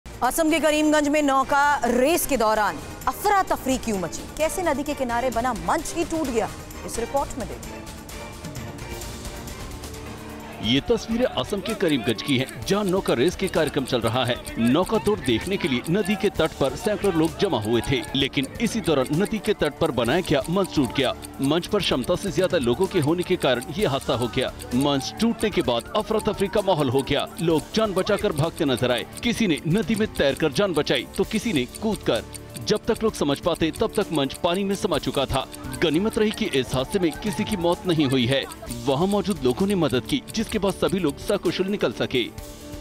Assam के Karimkanj में नौका रेस के दौरान क्यों मची अफरा तफरी? जानने के लिए देखिए ये रिपोर्ट
असम के करीमकंज में नौका रेस के दौरान क्यों मची अफरा तफरी..कैसे नदी के किनारे बना मंच टूट गया? आइए देखते हैं रिपोर्ट..
Why was there chaos during the boat race in Karimkanj, Assam? How did the stage built on the banks of the river break? Let's see the report..